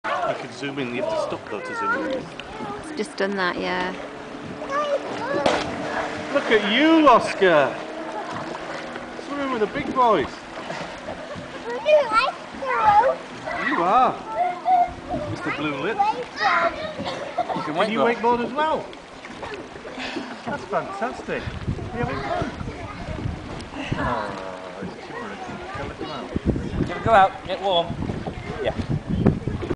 Oscar
Oscar swimming in the sea at Abersoch, August 2009